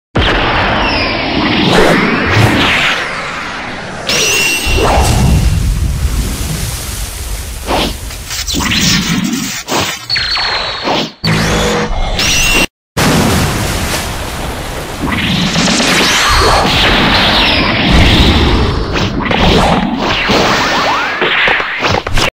PivotDBSuperZ - Goku Vs Moro IV Clip 1 Oficial
Hola gente, les dejo este clip de la próxima animación del canal.
muy pronto estaré dando detalles de la animación final.